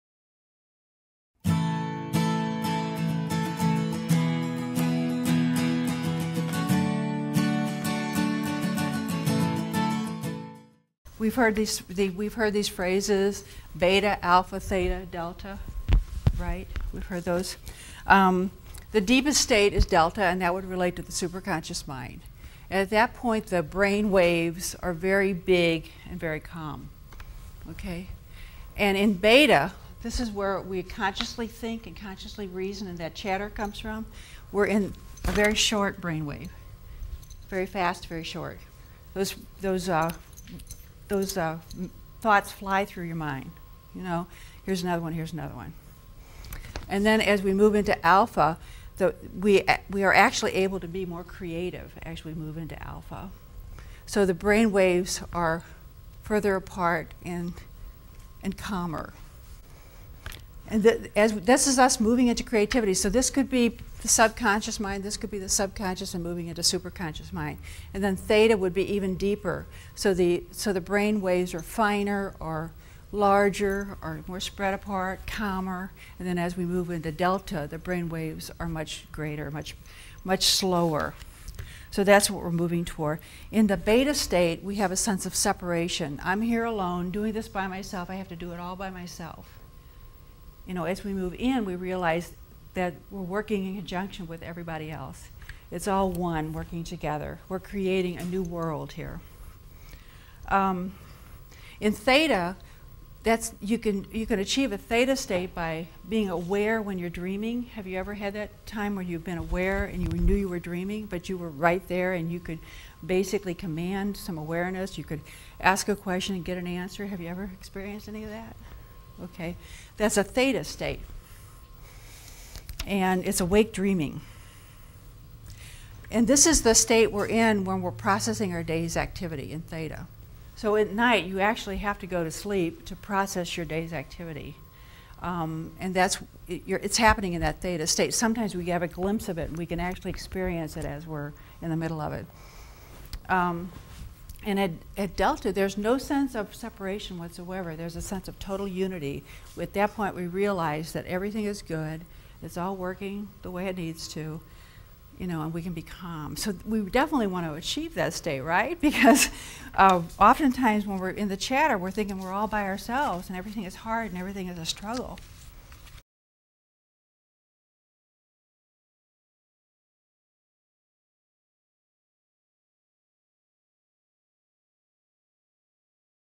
Brain Waves Explained
Personal Growth Consultant and Meditation Teacher Jean Walters explains the different functions of brain waves - Beta, Alpha, Theta and Delta.